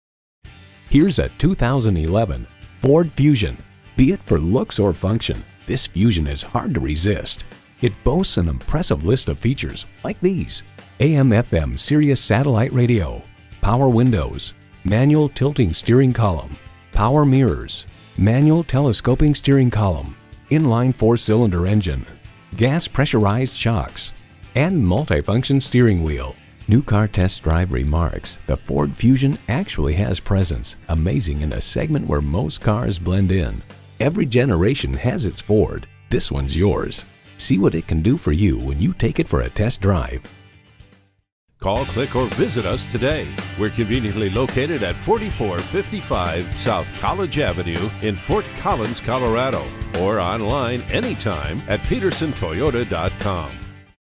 Used 2011 Ford Fusion Fort Collins Loveland, CO #1801371 - SOLD
Year: 2011
Make: Ford
Model: Fusion
Trim: SE
Engine: 4 Cyl. 2.5L
Color: Sterling Gray Metallic
Interior: Charcoal Black
Mileage: 115456
Stock #: 1801371
VIN: 3FAHP0HA0BR262136
D Recent Arrival!EXCEPTIONALLY CLEAN AND WELL CARED FOR VEHICLE!!!, ***CLEAN CARFAX!!!***.Awards:* 2011 IIHS Top Safety Pick

Address:
4455 S. College Ave